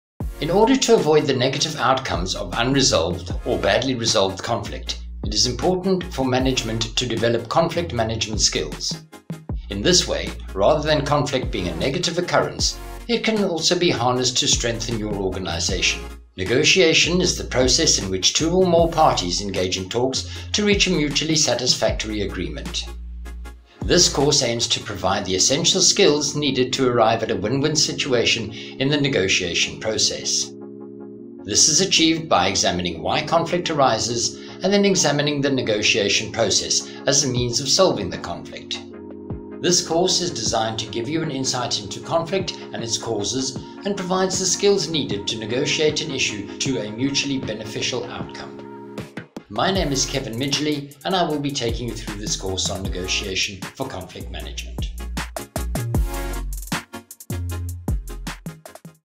Damelin Online | Negotiation for Conflict Resolution
Negotiation is the art of reaching an agreement where all parties benefit. In order to avoid the negative outcomes of unresolved or badly resolved conflict, it is important for management to develop conflict management skills. In this way, rather than conflict being a negative occurrence, it can be harnessed to strengthen an organisation.

to find out more about our course offerings.